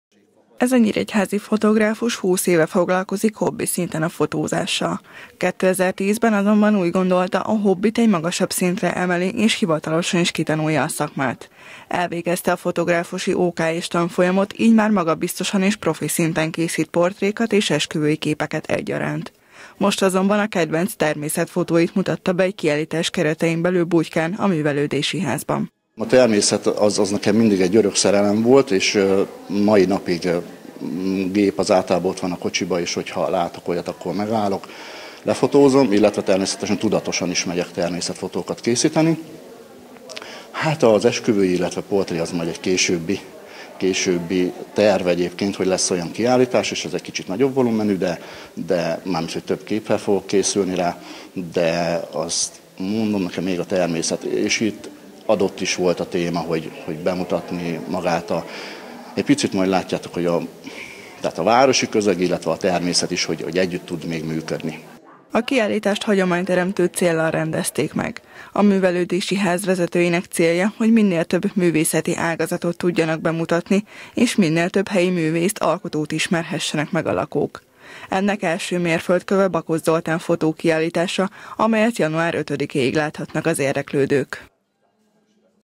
Fotókiállítás Butykán - Kölcsey Tv
Hagyományteremtő céllal nyílt fotókiállítás Butykán a Művelődési házban. Bakos Zoltán nyíregyházi fotográfus mutatta be néhány természetfotóját, amelyet a közönség január 5-ig láthat.
A művelődési ház célja, hogy minél több helybéli művész mutathassa be alkotásait minél szélesebb témakörben.